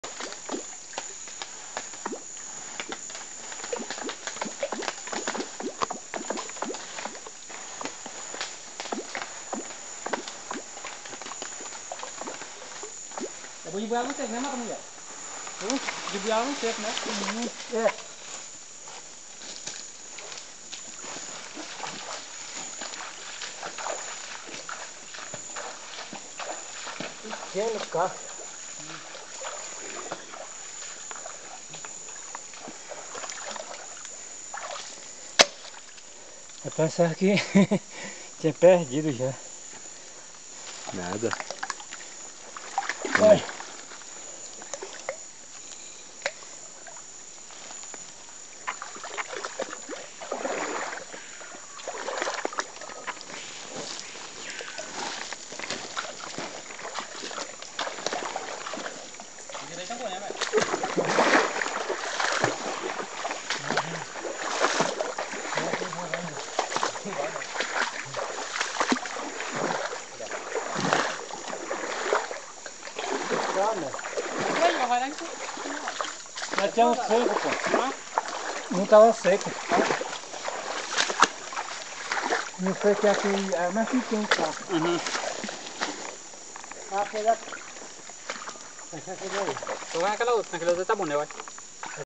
Collecting Acai
Collecting fruit from the acai palm (Euterpe oleracea), Medio Jurua, AM, Brasil